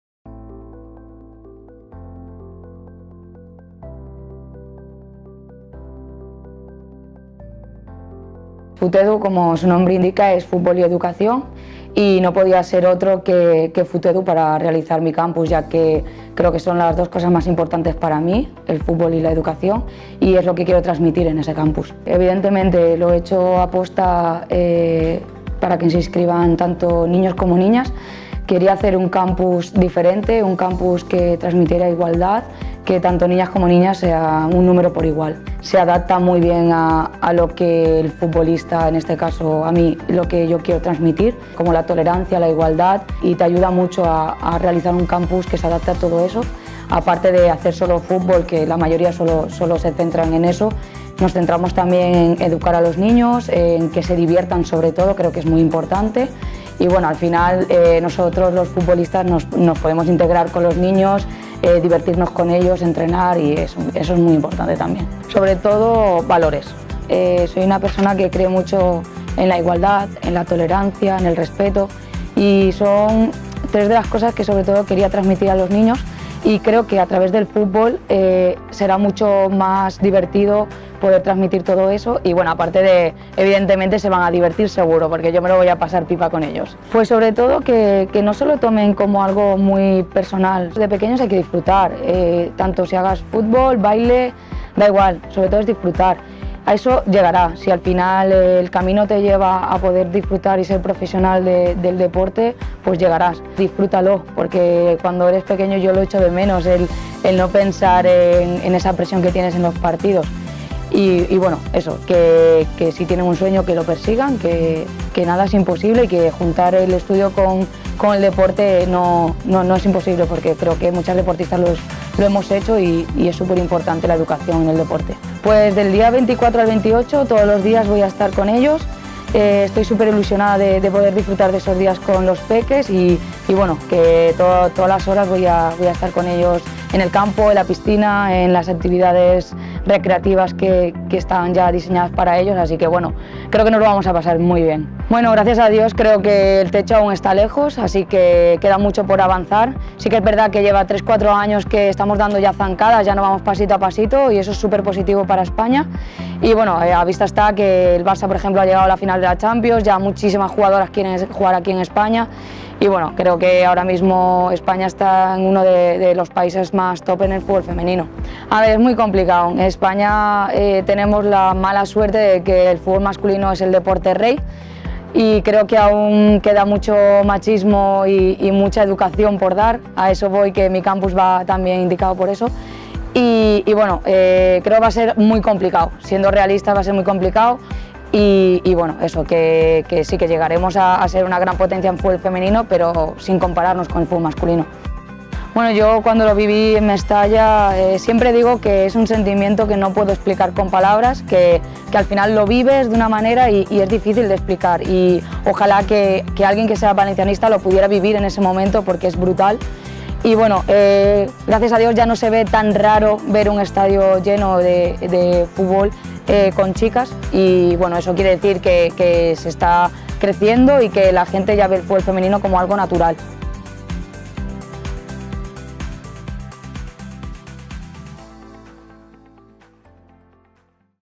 Entrevista Mari Paz Vilas | Campus coeducativo 2019.
La jugadora del Valencia CF, presenta su campus coeducativo de la mano de Futedu. Un proyecto educativo por la igualdad y los valores del deporte.